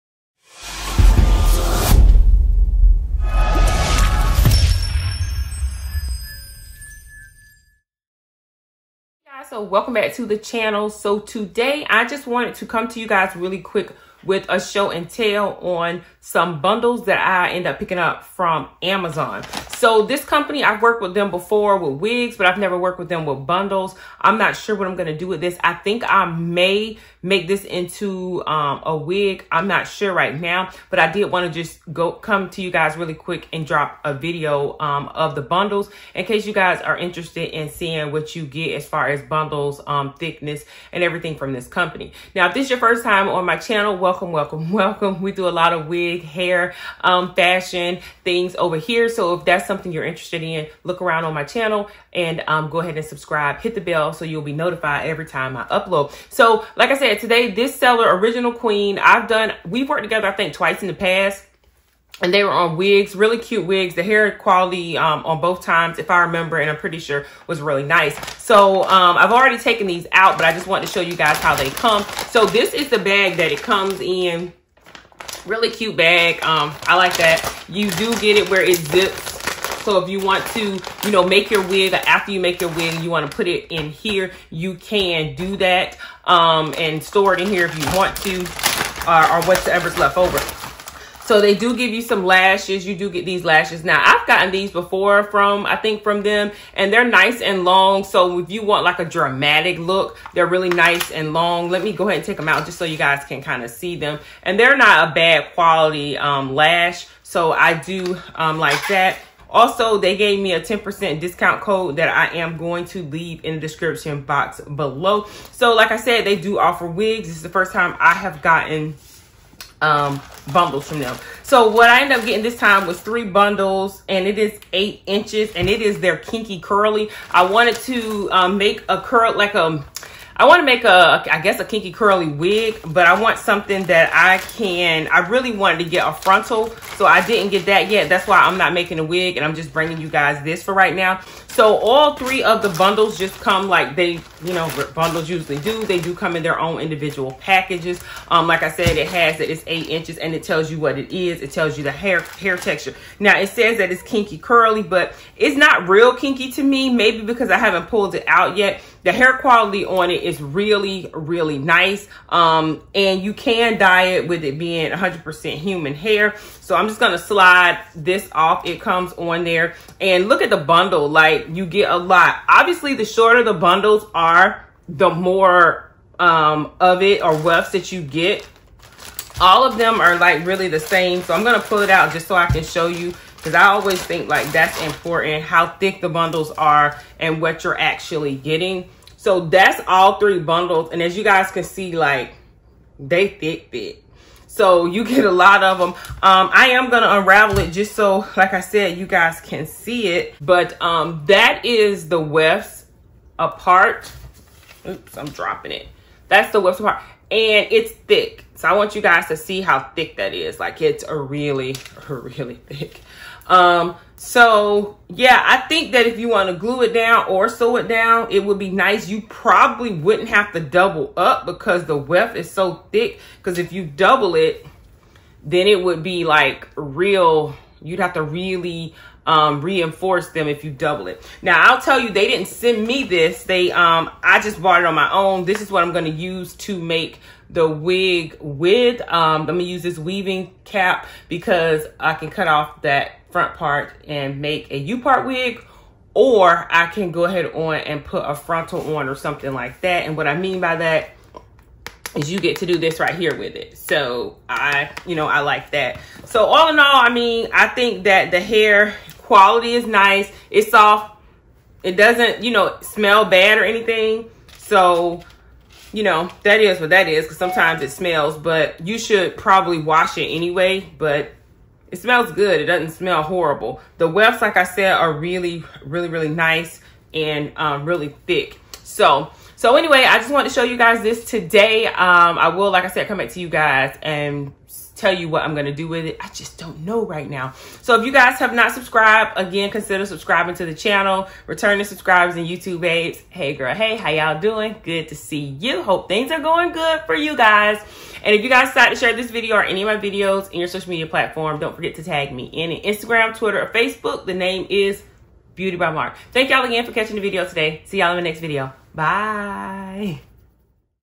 $36 AMAZON HUMAN HAIR BUNDLES | ORIGINAL QUEEN HAIR | SHOW & TELL | DISCOUNT CODE | BEAUTIEBYMARK
This video is about Original Queen 100% Brazilian Unprocessed Virgin Kinky Curly Human Hair Weave 3 Bundles Deep Curly Hair Extensions Mixed Length 8 8 8inches by Original Queen

LINK TO PUCHASE

DISCOUNT CODE: OQHAIR17